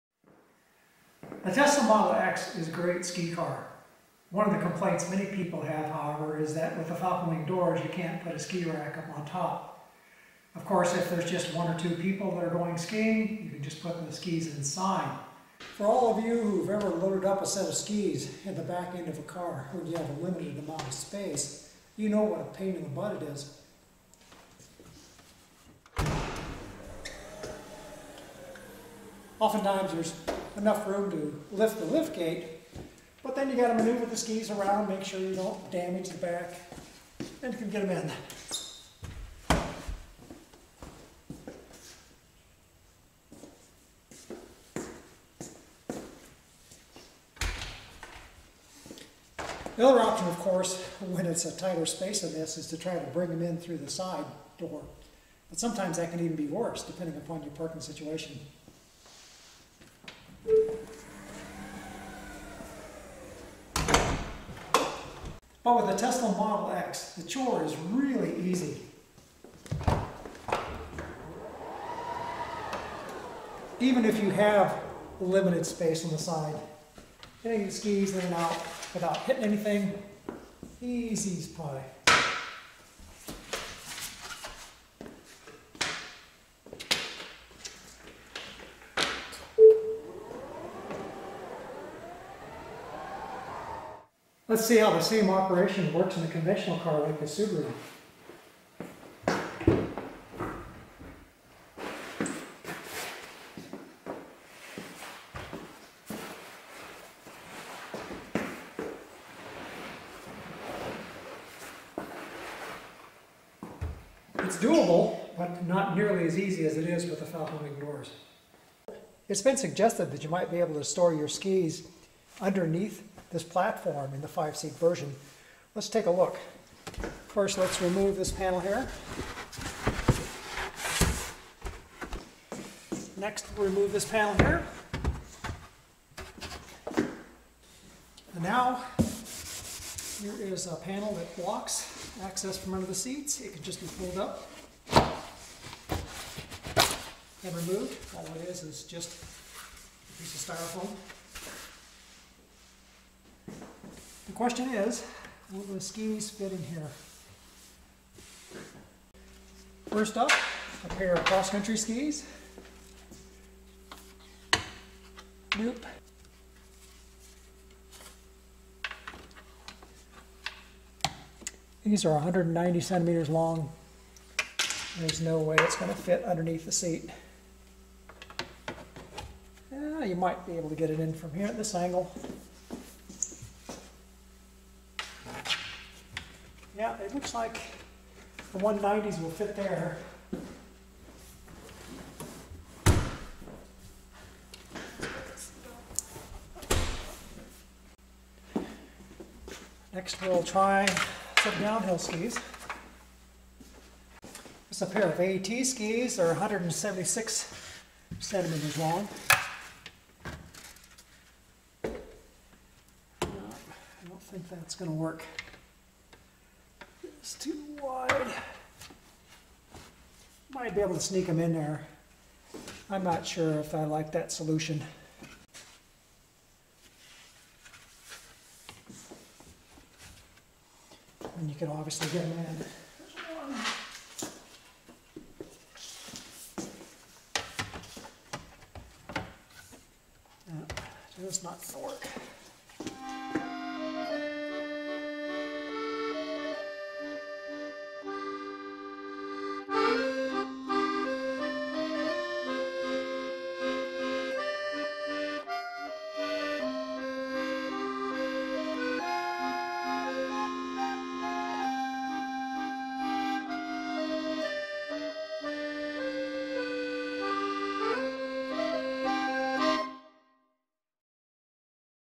Putting skis inside a Tesla Model X 5 seat
You cannot put a conventional roof ski rack on top of a Tesla Model X.  Tesla sells a ski rack that can be attached to the hitch receiver.   I transport my skis inside the Tesla with one of the back seats folded.  I protect the seat back with a towel and a ski bag.  The falcon-wing doors make it easy to get the skis in and out of the car, much easier than any other car I have owned.